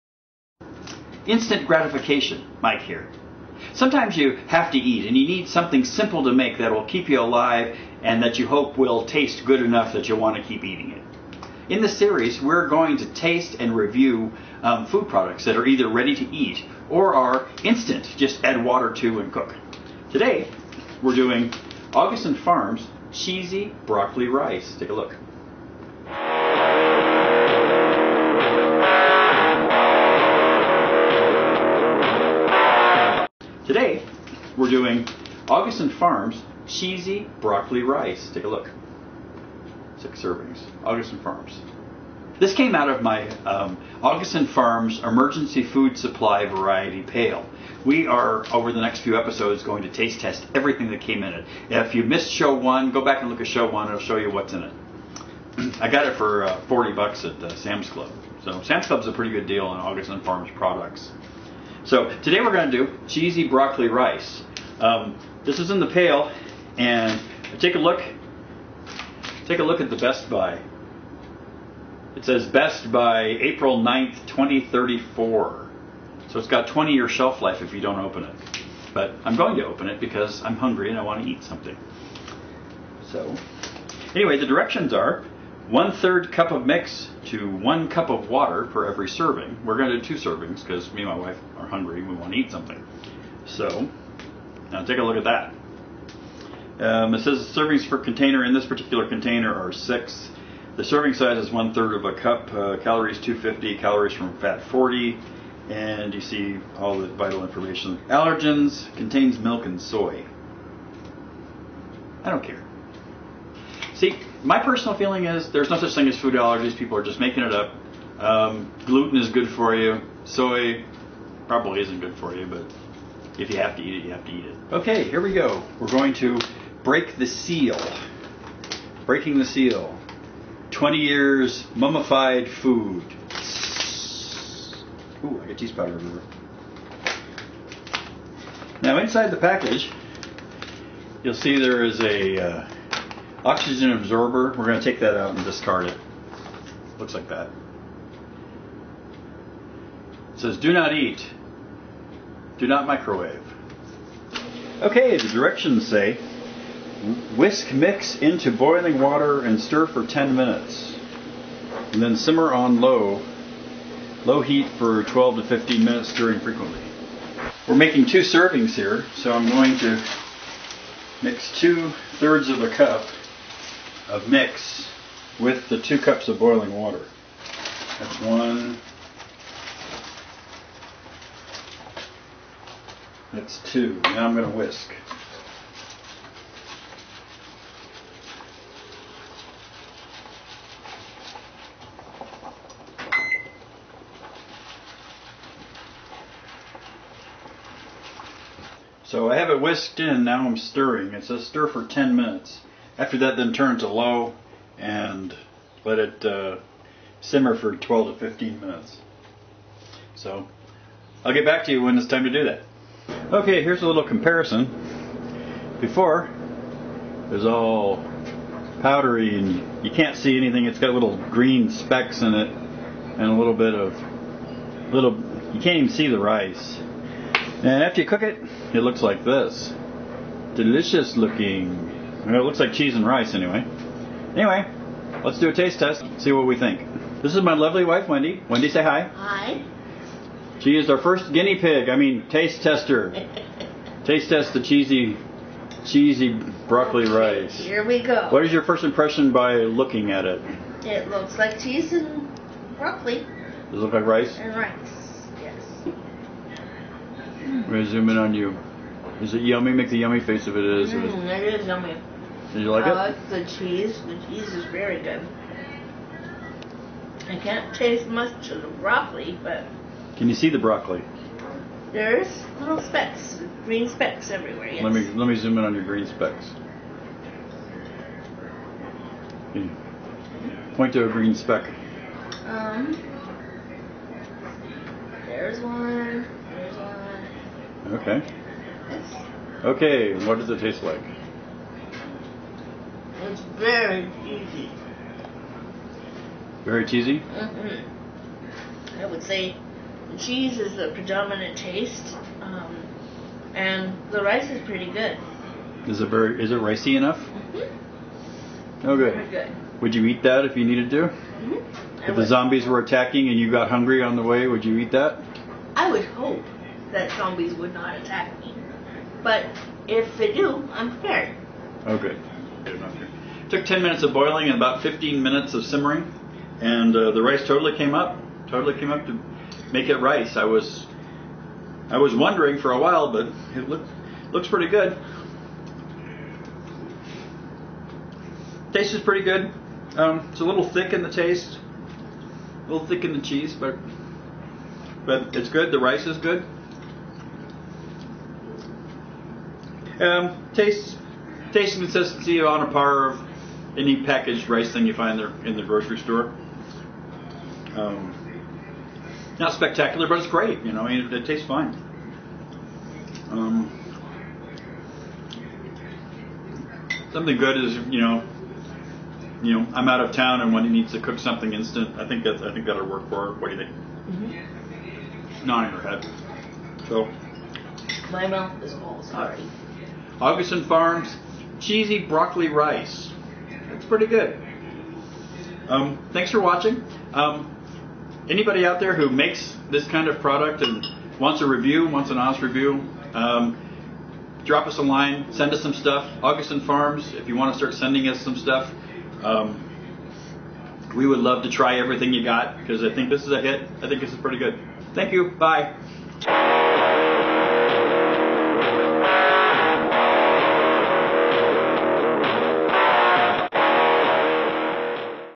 Instant Gratification Episode 2 - Augason Farms Cheesy Broccoli Rice Review
Instant Gratification! In this series we look at ready to eat and Instant food - just add water and or cook.  In this episode we taste test Augason Farms Cheesy Broccoli Rice from the Augason Farms Emergency Food Variety Pail.  Over the next several episodes we'll be taste testing everything in the bucket.